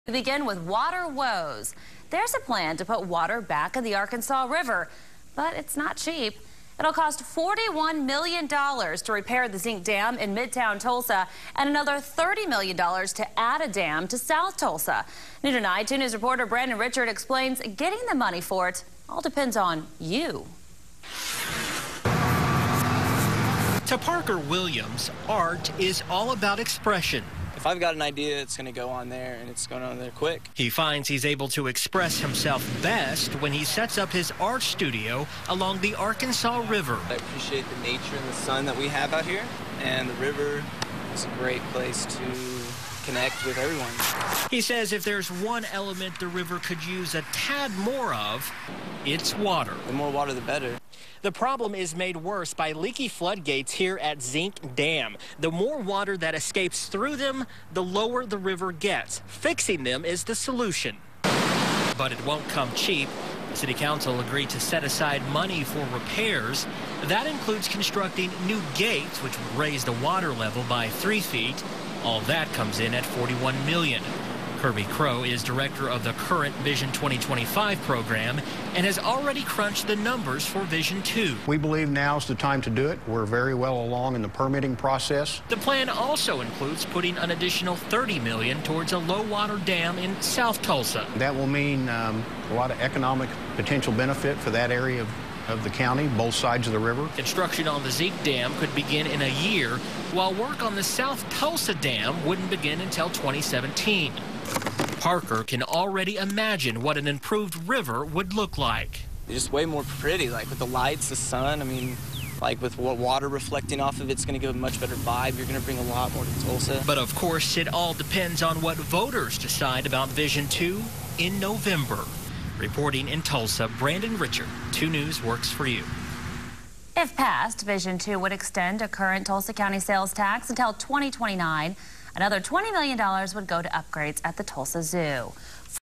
Councilors agree to spend $71 million on Arkansas River
2News at 10pm.